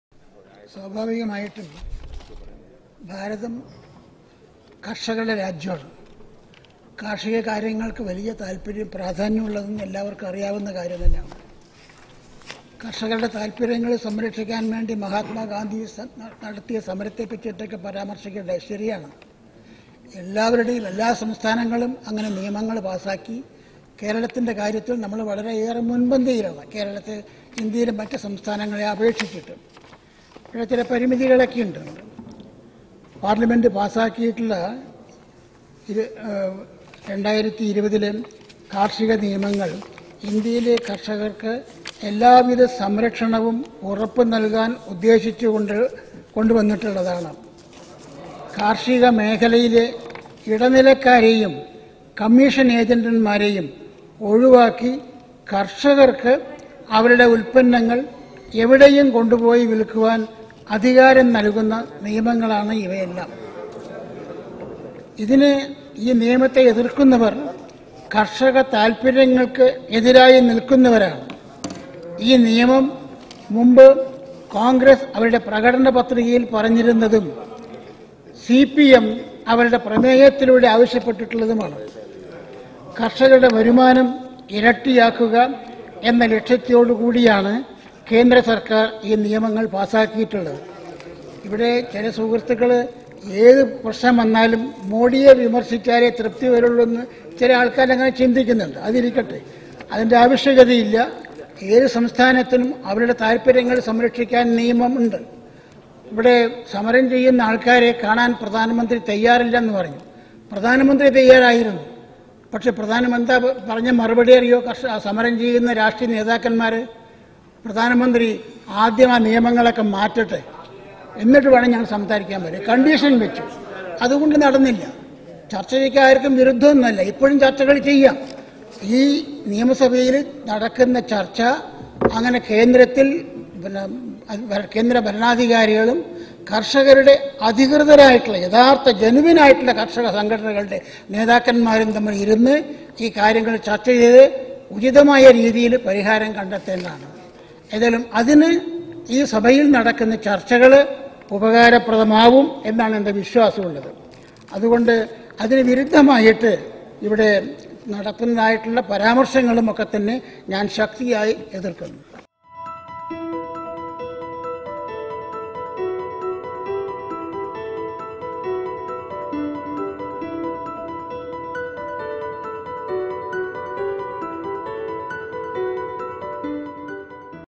കാര്‍ഷിക നിയമങ്ങളെ എതിര്‍ക്കുന്നവര്‍ കര്‍ഷക വിരുദ്ധര്‍: ഒ രാജഗോപാല്‍
15th KLA , 5th session കാര്‍ഷിക നിയമങ്ങളെ എതിര്‍ക്കുന്നവര്‍ കര്‍ഷക വിരുദ്ധര്‍: ഒ രാജഗോപാല്‍